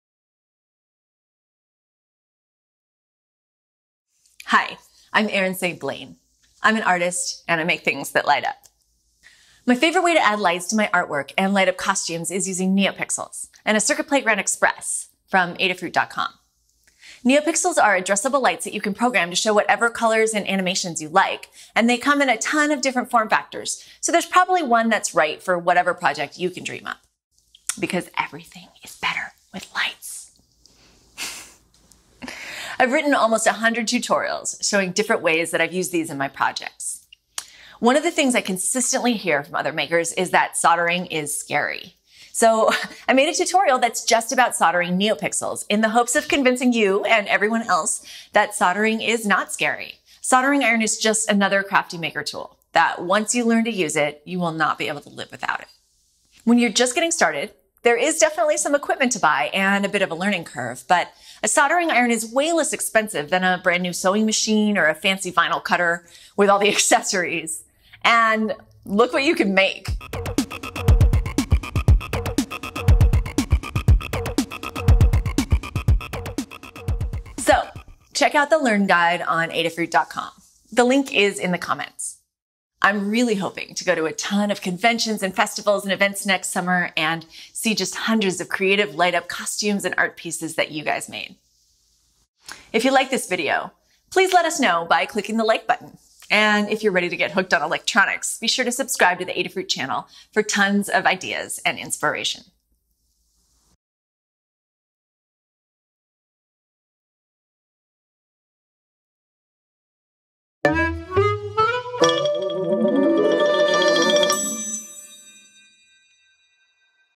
New Learn Guide: Soldering NeoPixel LEDs for Costumes & Artwork
This guide is for beginners who are ready to take the next step in DIY electronics: putting that soldering iron to use to add NeoPixel or other addressable LED lights to your project.

You don't have to be able to solder to use NeoPixels! Here is a beginner tutorial that shows how to use NeoPixels with no soldering:

However, there are times when soldering is really the best option. If you want to use NeoPixel rings, or individual pixels, or create more elaborate or complicated designs with multiple strips, there's really no way to do it without soldering.

The good news: it's not as hard as you think! There are a fair number of tools to acquire, but once you've got your station set up, a whole world of creative possibilities will open up for you.

In this guide, we'll recommend some tools and equipment that every DIY electronics maker should have in their workshop. Then, we'll go over the techniques and skills needed to get a good solder connection, and how to secure your connections it so they are sturdy and reliable.

Finally, we'll cover some techniques for repairing broken strips, and troubleshooting help for when your project just won't behave the way you want.